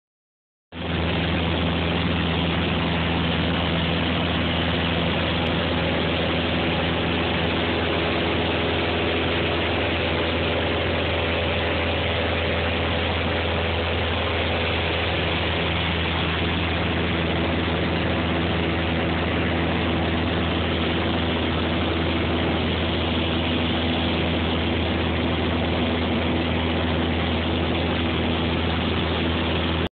Steven's pass at 10,500 ft in a Cessna 172
Coming home from eastern Washington over the Cascade Mountains, somewhere above Icicle Canyon.